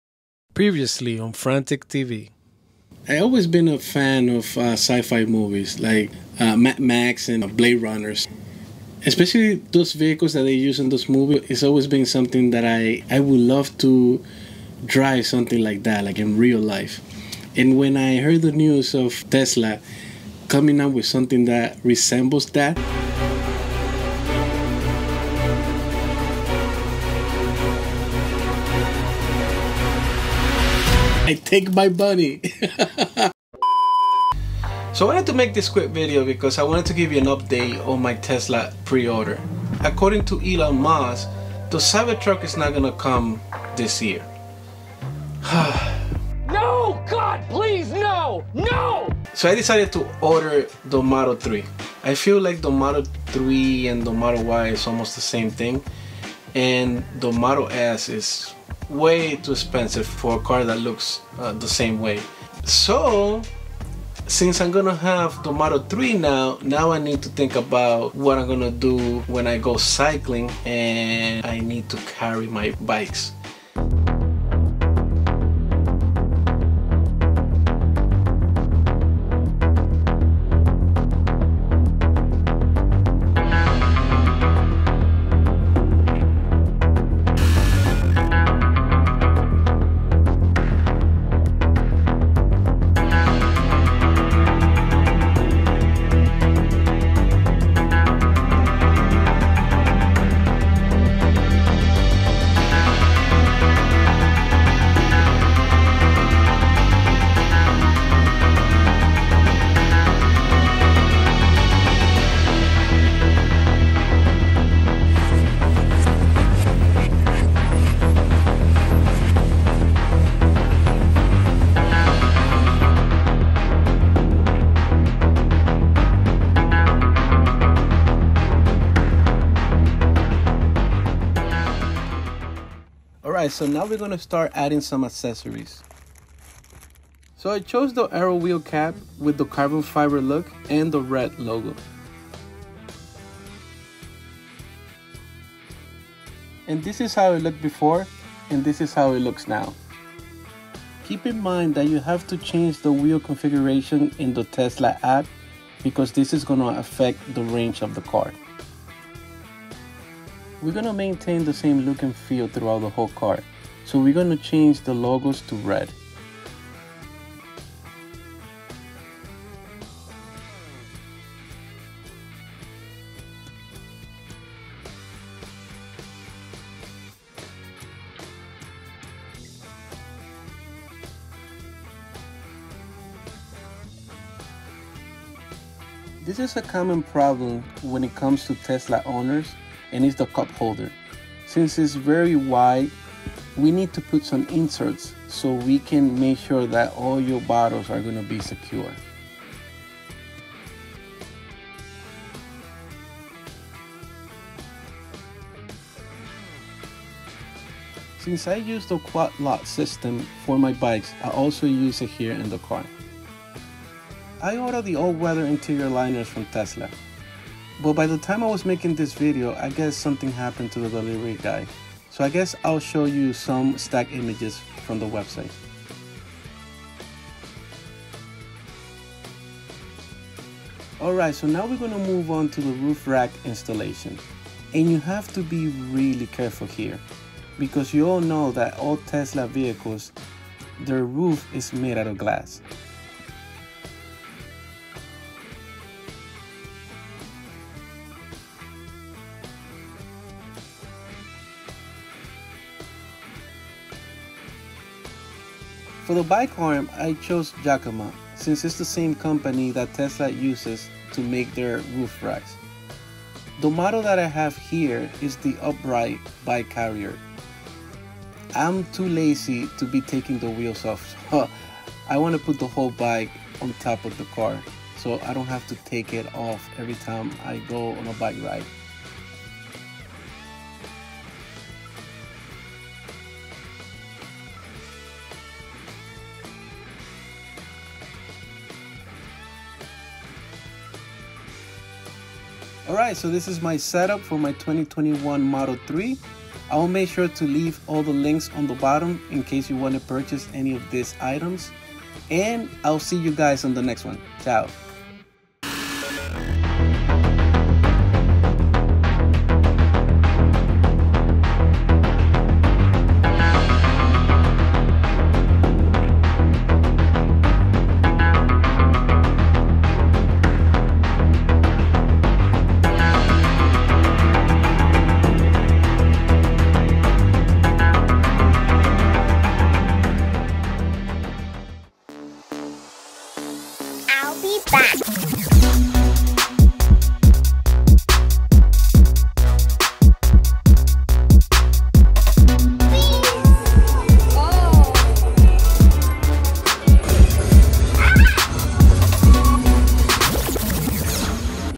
Tesla model 3 Roof Rack System and Accessories
Frantic TV Hosted by Felix Mariano

1 - YAKIMA - HighRoad Wheel-On Mount Upright Bike Carrier for Roof Racks

3 - Tesritar Tesla Cup Holder

4 - Logo Decal Wrap for Tesla Model 3 (Gloss Red)